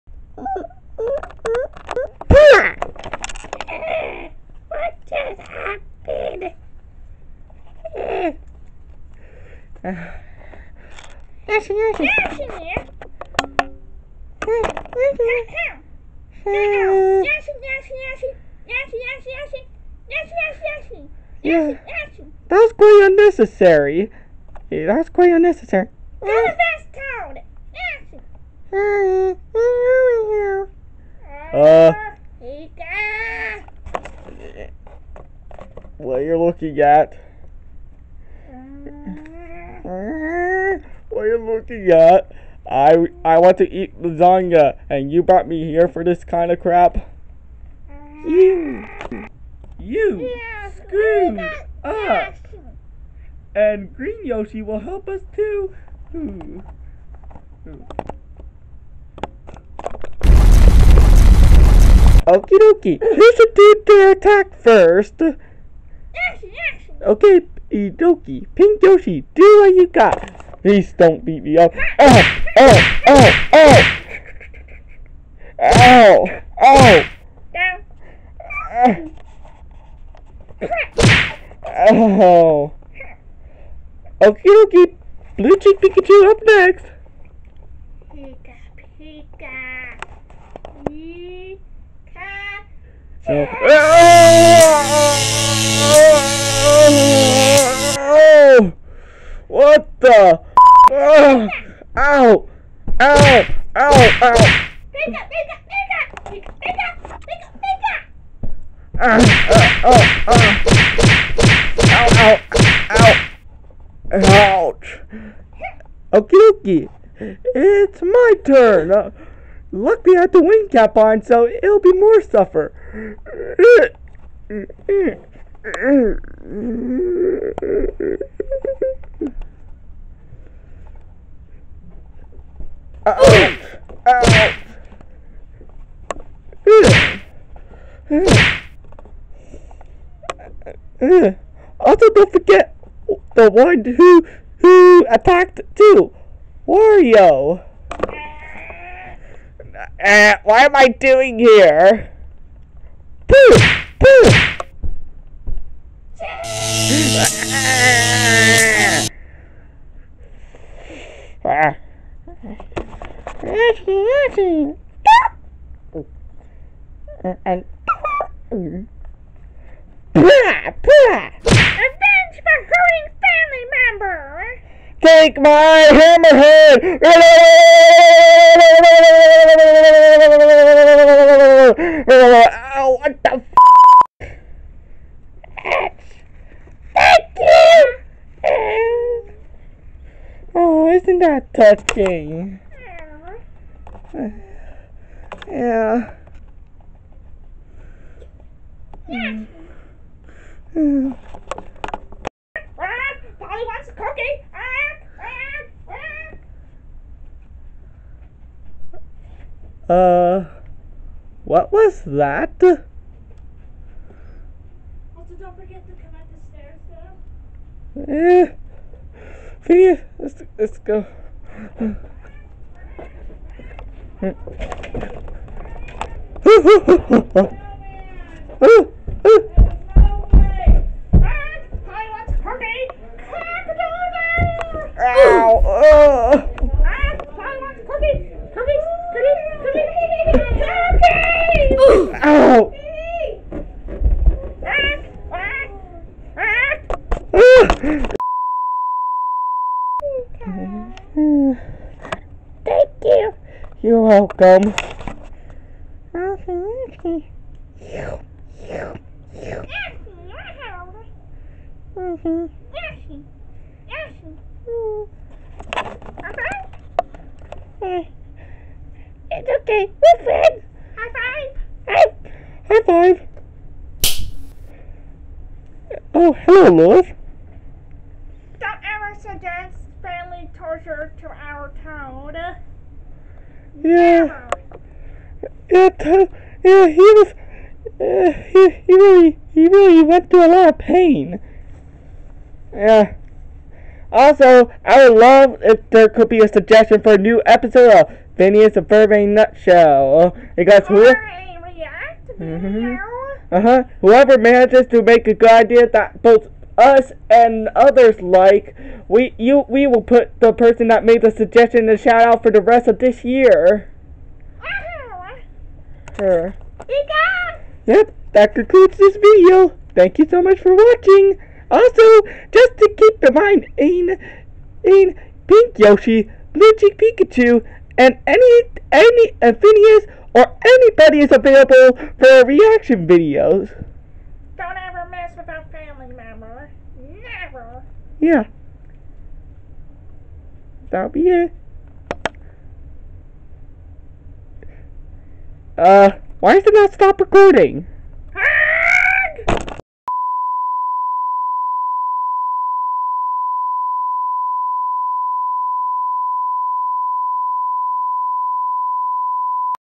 Seth Horst Plush Short: Payback to Garfield and Wario
This was suggested by my Sister named Raven, Also the mailman Toad is actually the Toad that wears the golden fabric vest and he owns the Pikachu that has blue cheeks instead of red ones. I hope you enjoy this video. There will be more videos soon.